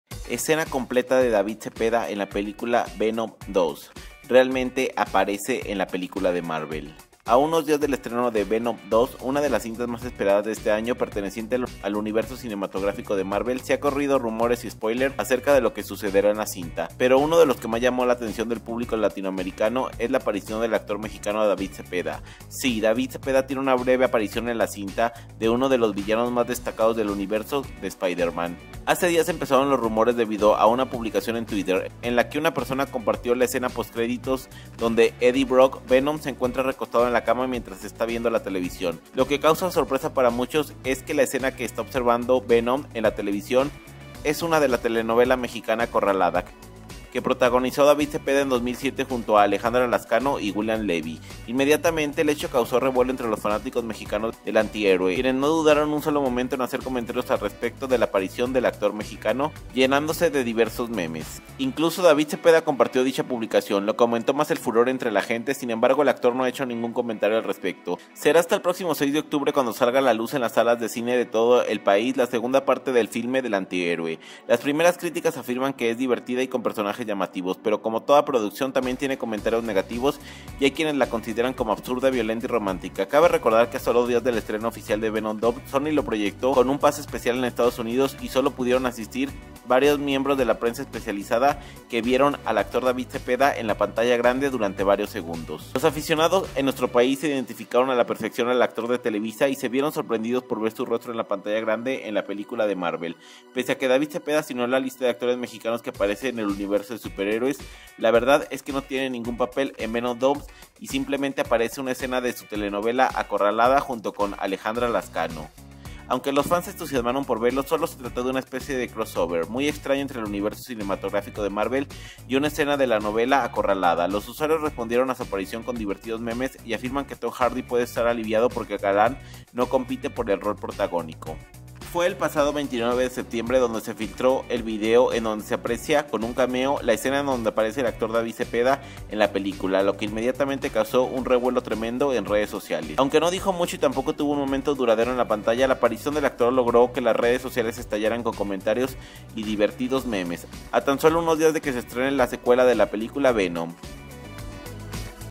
Actor mexicano David Zepeda sorprende al aparecer en “Venom 2”: “no me lo esperaba”
Zedepa realmente si sale en Venom 2